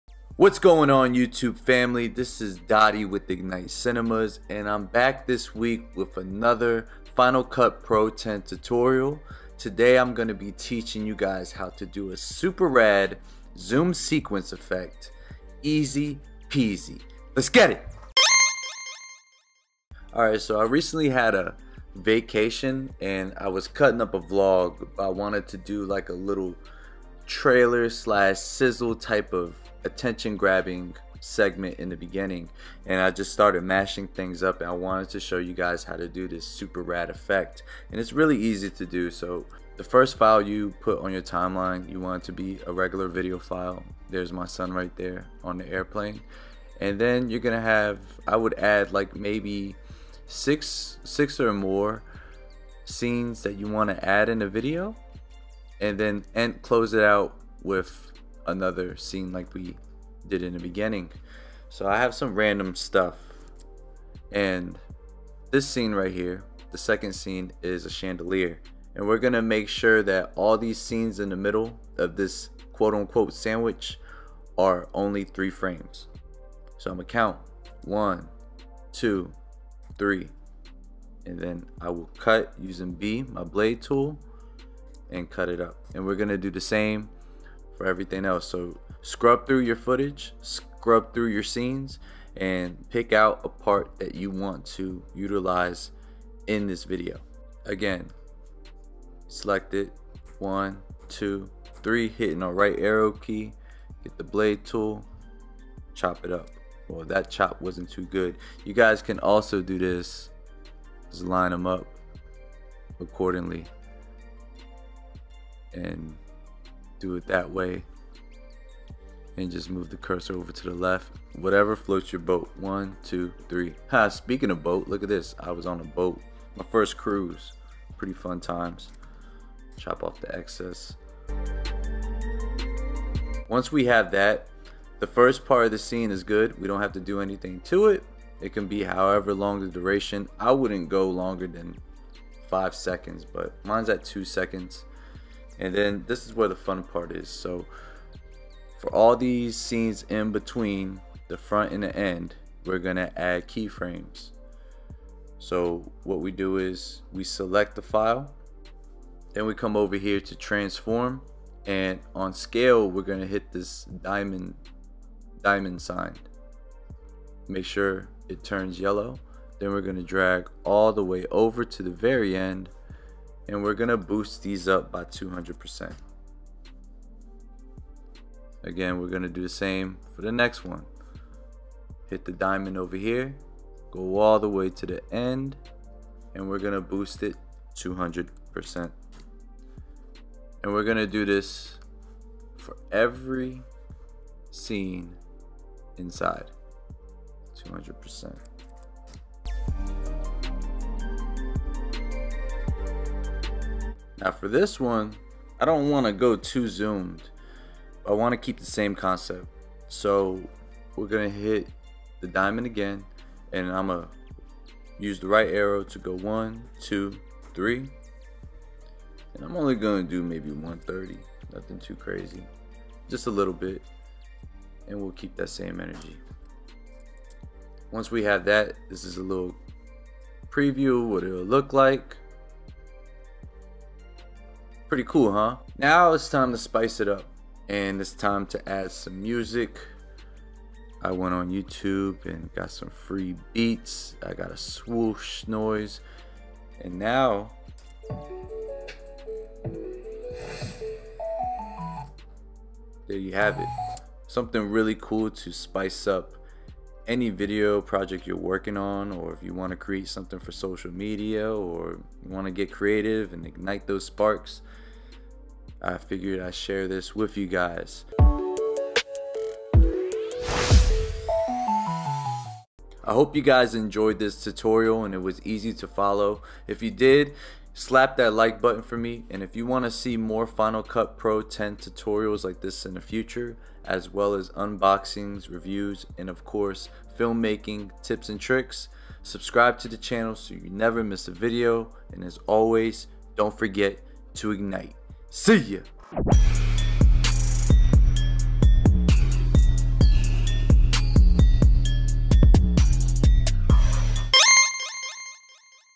ZOOM Sequence Effect - Final Cut Pro X Tutorial
Want a rad & simple effect to sizzle up your video projects? Check out this Zoom Sequence Effect Tutorial using Final Cut Pro X & IGNITE your video to the next level.

Don't forget to IGNITE !